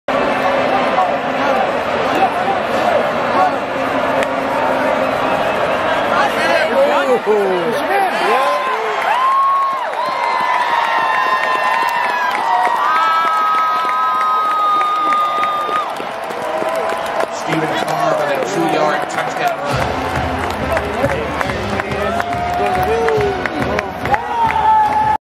trojancandy.com: See USC's Stephen Carr Score his Touchdown in the 2019 USC vs Cal game in Berkeley
At trojancandy.com: See USC's Stephen Carr score his touchdown in the 3rd quarter of the 2019 USC vs Cal game in Berkeley.  Read the entire story and see more pictures and videos